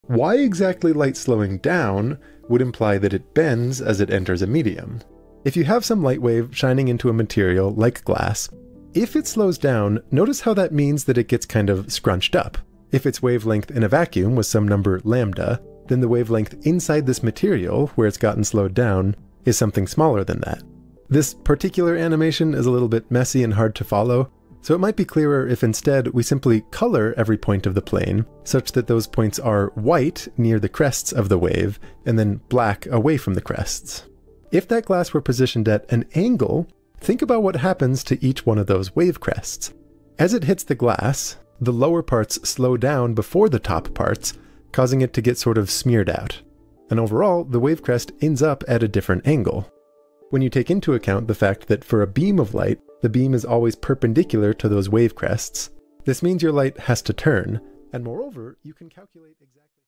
Why does light slowing imply a bend? (Beyond the tank/car analogy)
A link to the full video is at the bottom of the screen.

That video answers various viewer questions about the index of refraction.

Editing from long-form to short by Dawid Kołodziej